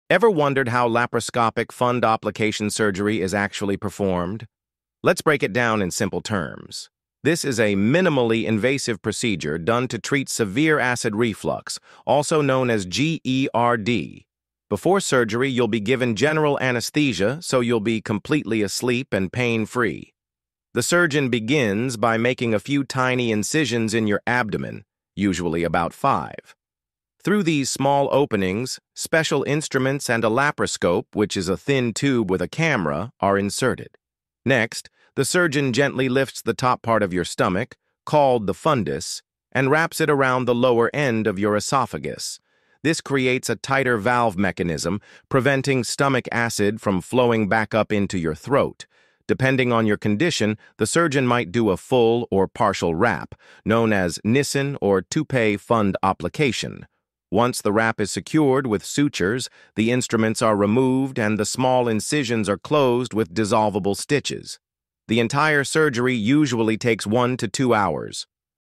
How Laparoscopic Fundoplication Surgery Is Performed| Step-by-Step procedure
Laparoscopic Fundoplication is a minimally invasive surgery used to treat acid reflux (GERD). In this video, learn how surgeons perform the procedure using small incisions and a laparoscope to wrap the upper part of the stomach (fundus) around the esophagus, preventing acid reflux.
👨‍⚕️ Watch the complete step-by-step surgical process explained visually!